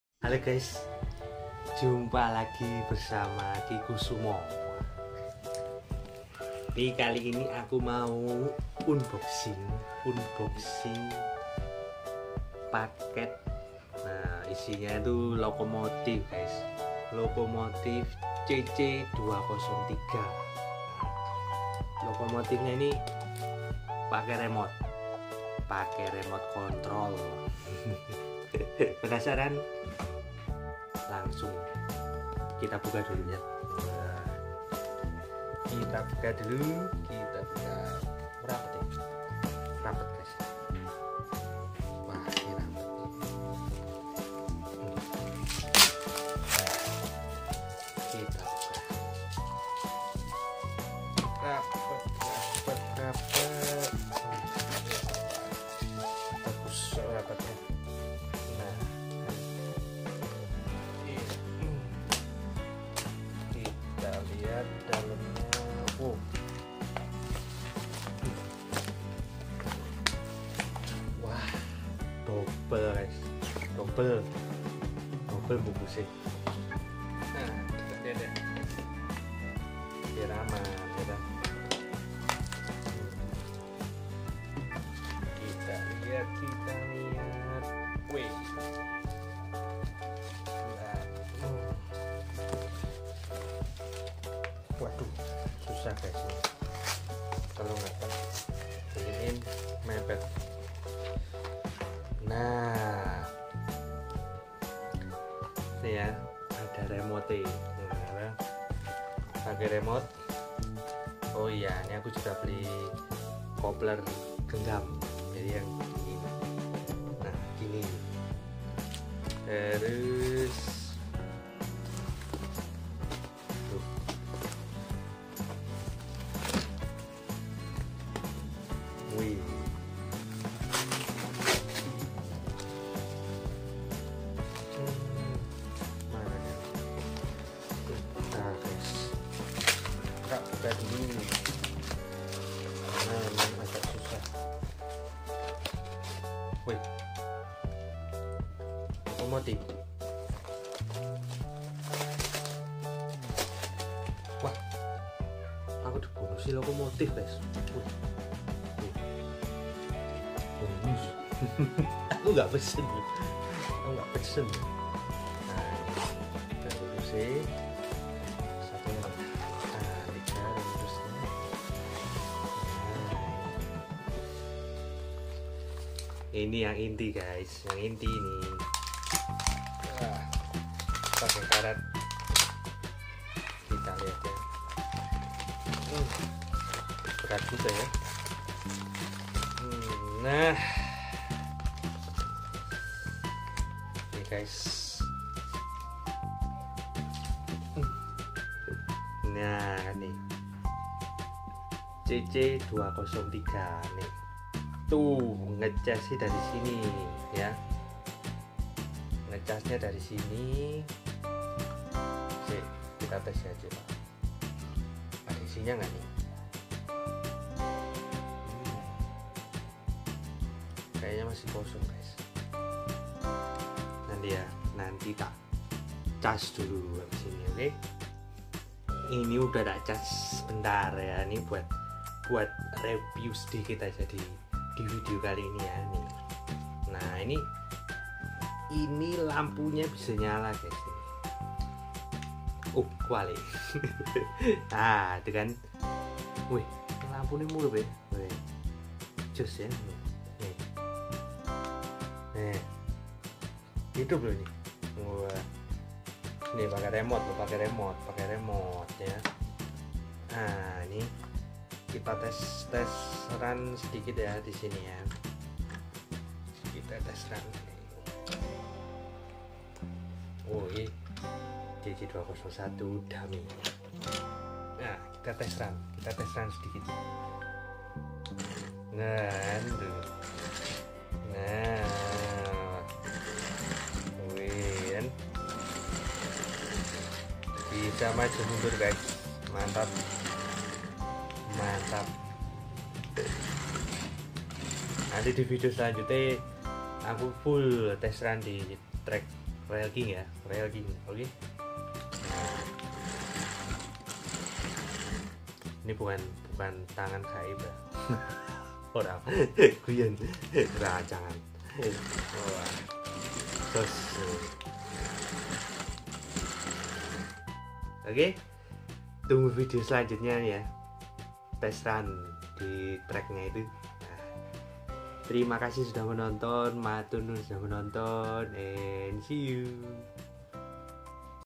UNBOXING n REVIEW Lokomotif RC CC203 || Miniatur Kereta Api Indonesia
akhirnya RC lokomotif cc 203 ku dateng, maju mundur & lampunya di kontrol pake remote...bagussssss😁😁😁😁